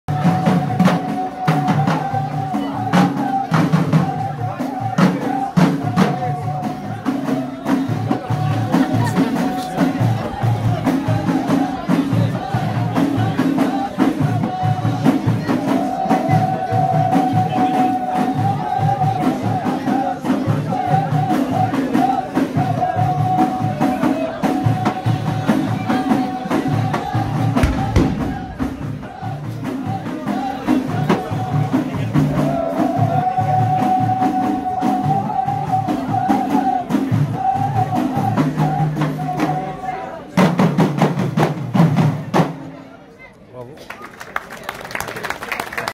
Femme amazigh maestro. Morocco
Friday, July 19, 2019. Festival de Missour.
Père et fille.

Première femme Maestro Ahidus, also sometimes called ahidous, haidous, tahidoust or hidoussi, is a style of collective performance in Morocco. It is the traditional dance in many amazigh tribes and is known to be the favorite entertainment of these tribes